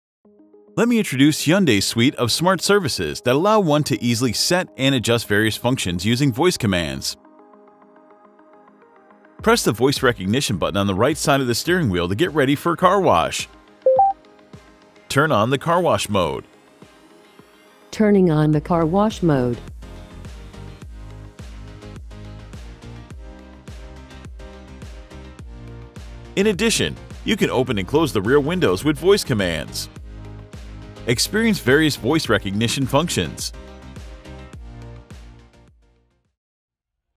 HYUNDAI HOW-TO | Voice Recognition
Model shown in video is for demonstration purposes only. Availability and functionality on your vehicle may vary.

Hyundai offers new infotainment system ccNC (connected car Navigation Cockpit), depending on your model. Watch this video and learn how to use voice recognition function provided by ccNC infotainment system.